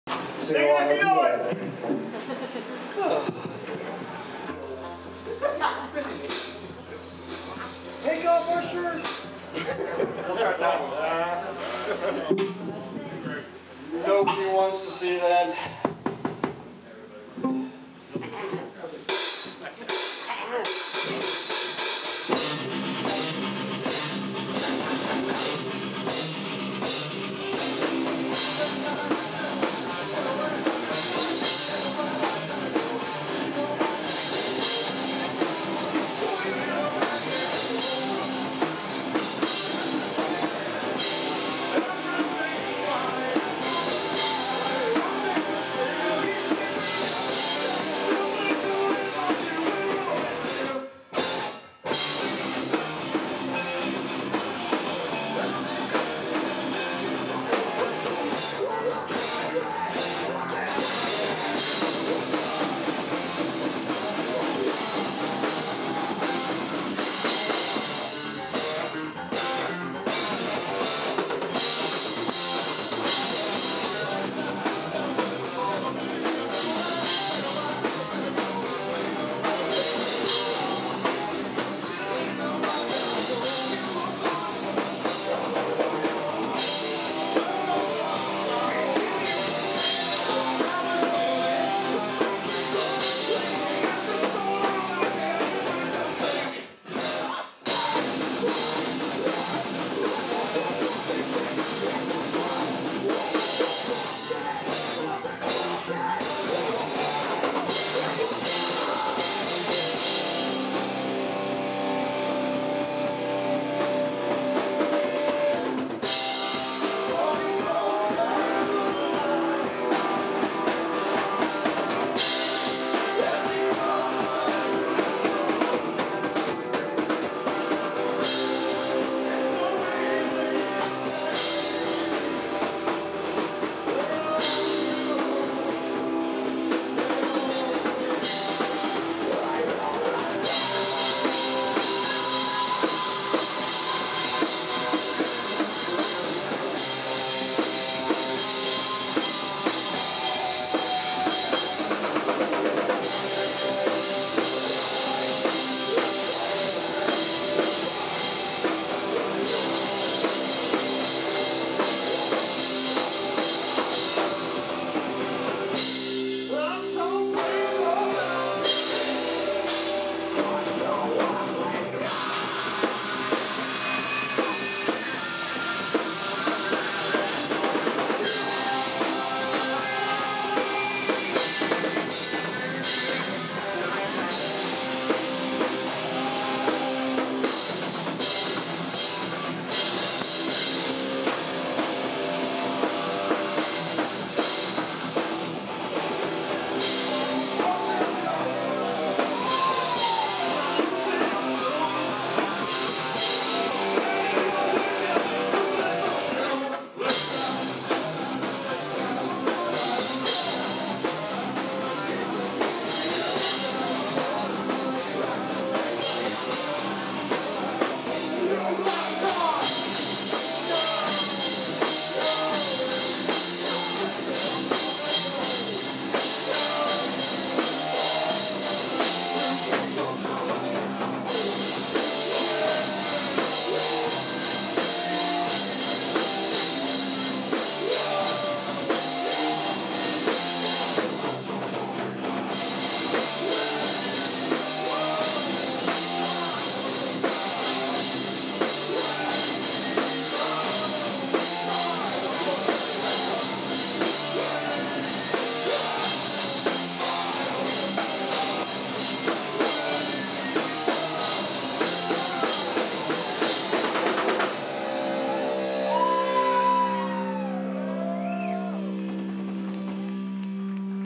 Disrupt The Paradigm 5/18/19~6
Disrupt The Paradigm live at Toot's Tavern, Crockett, CA. 5/18/19